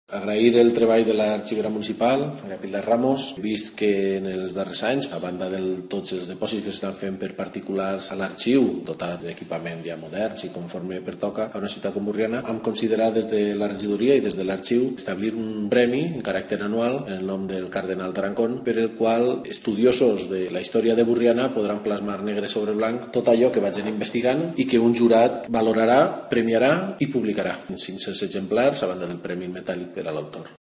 Burriana instituye el Concurso de Investigación de historia local "Cardenal Tarancón"
El alcalde de Burriana, José Ramón Calpe, ha firmado esta mañana el decreto de aprobación de las bases del primer Concurso de Investigación de Historia Local "Cardenal Tarancón", que el Ayuntamiento ha instituido para fomentar el estudio de temas relacionados con la ciudad y su historia y que prevé un premio en metálico de 500 euros y la edición de 500 ejemplares del trabajo seleccionado.